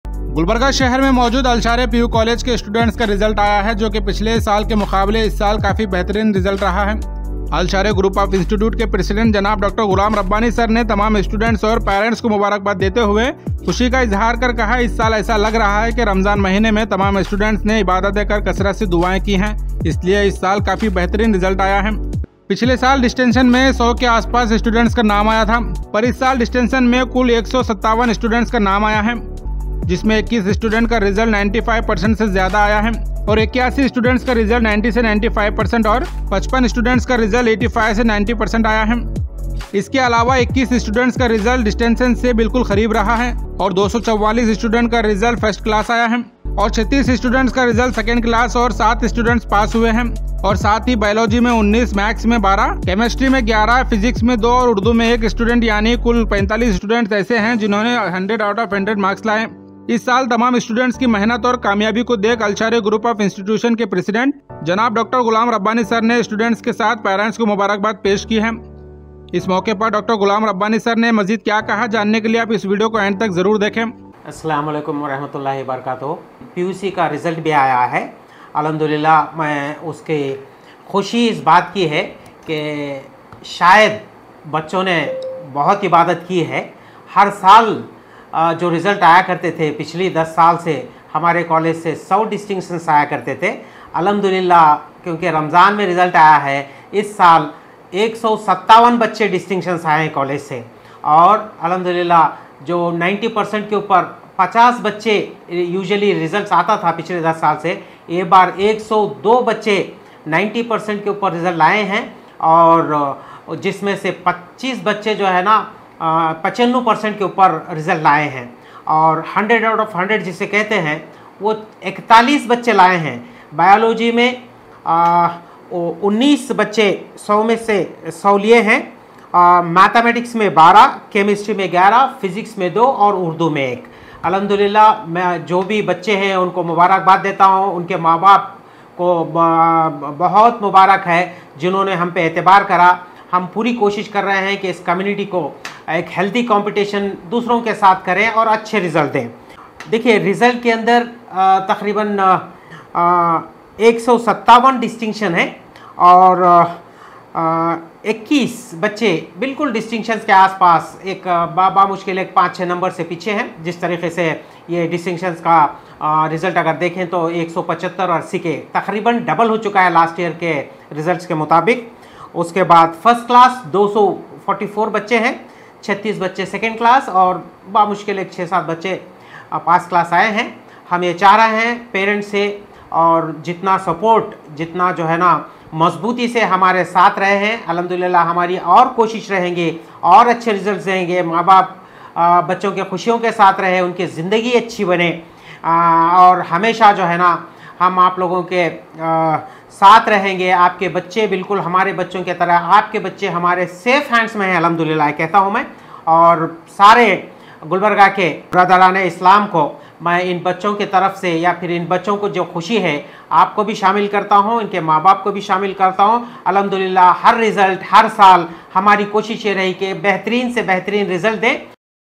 Students of Al Sharay Group of Institutions have secured the top spots in the PUC II Year exams.
Students of Al Sharay Group of Institutions have secured the top spots in the PUC II exams. Congratulations to them for their remarkable performance as brilliant scholars